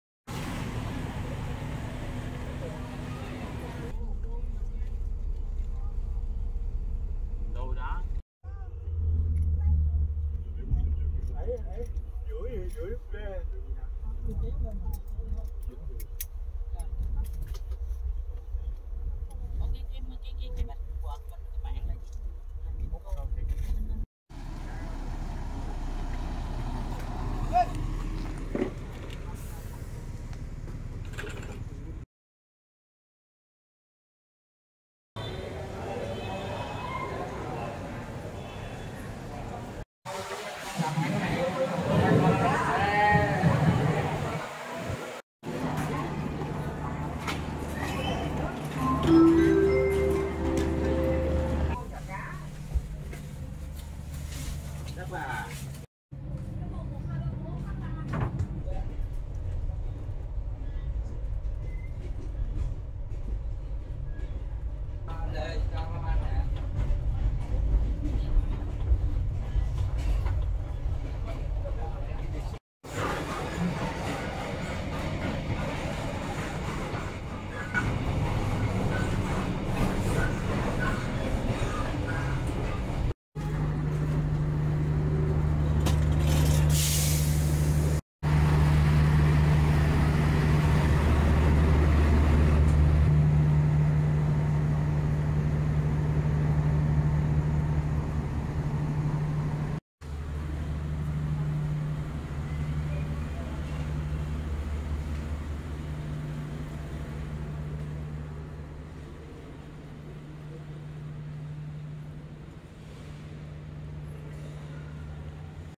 【ベトナム】ニャチャン-ホーチミン間を夜行列車、夜行バスで移動【2023年10月】
【撮影日】2023/10/02(月)、2023/10/05(木)
【撮影場所】ベトナムホーチミン、ニャチャン
2023年10月ベトナムニャチャンに旅行に行きました。
ニャチャンへはホーチミン経由で移動。行きは夜行バス、帰りは夜行列車を利用しました。

・撮影：スマホ(Vivo X90)
・編集：davinci resolve

■ブログ記事
【ベトナム】ニャチャン-ホーチミン間を夜行列車、夜行バスで移動